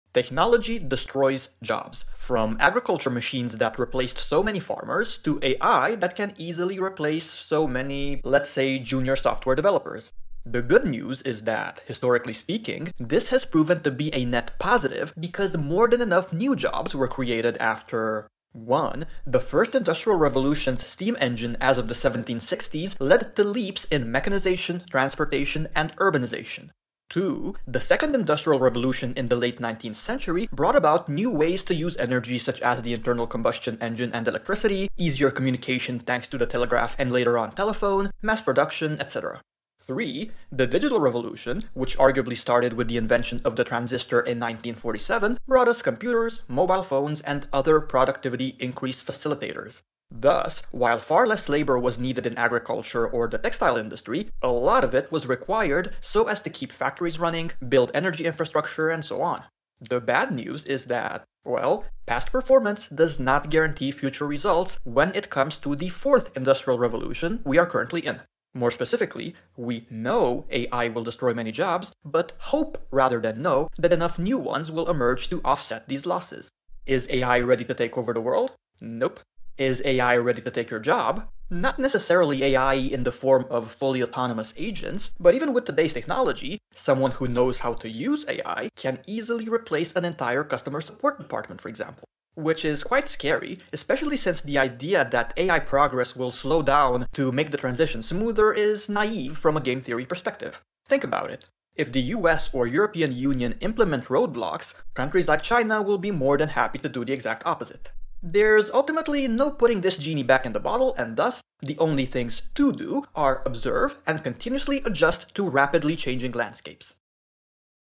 How AI Might Take Your Job Explained in One Minute
When analyzing this topic, most people are afraid of the wrong thing, of something along the lines of fully autonomous AI agents completely taking over. Which, for the foreseeable future and for the overwhelming majority of jobs, is VERY unlikely.

Does this mean your job is safe?

I'm afraid not.

Because as this video makes clear, it's not AI robots like you see in SciFi movies that you should be afraid of but rater scenarios such as a fellow human being who knows how to use AI taking your job... along with that of all your coworkers.

Can we at least be certain that just like with the previous Industrial Revolutions, more than enough new jobs will be created this time around as well?

Yet again, the answer is no.

Past performance does not guarantee future results, as many of you already know, so we can only HOPE that is the case. But since hoping is not exactly the wisest strategy, the name of the game is also preparing for the worst and starting to think about how you can start leveraging AI to become better and better at whatever it is you currently do.

If you can't beat them, join them!